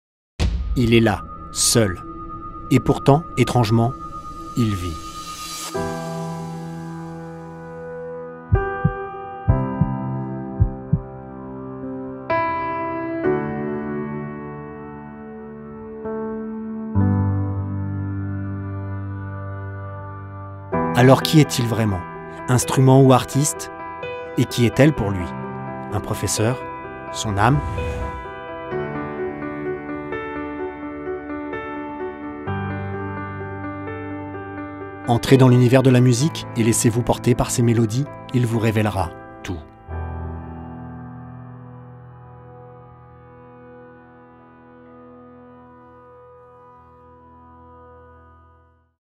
Un piano virtuose | BA2Sc #16
🎬 Si la recherche était un film, à quoi ressemblerait sa bande-annonce ?
Au Box Office cette semaine :  laissez vous bercer par la douce musique d'un piano... la danse gracieuse des doigts du pianiste qui s'efface... Et pourtant... la mélodie continue de se jouer sur les touches noires et blanches de l'instrument... Mais comment est-ce possible ?

Production et communication : CNRS Images & CNRS.